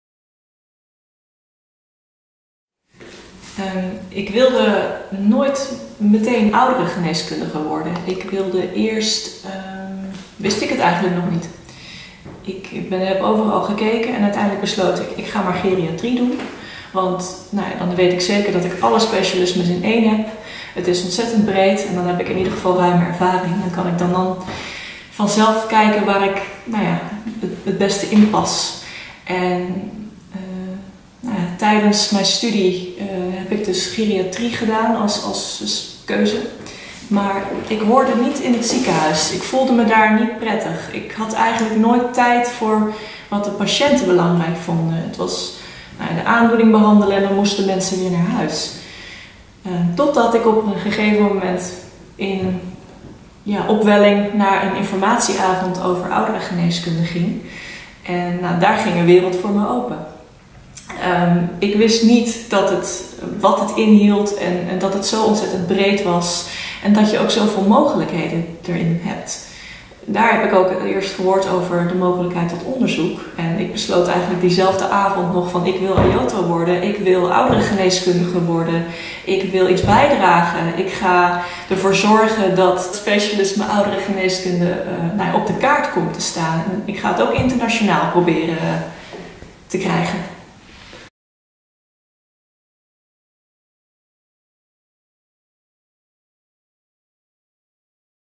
Aioto Karin Pouw vertelt • Ouderengeneeskunde.nu
"Ik hoorde niet in het ziekenhuis". Aioto Karin Pouw over haar werk als ouderengeneeskundige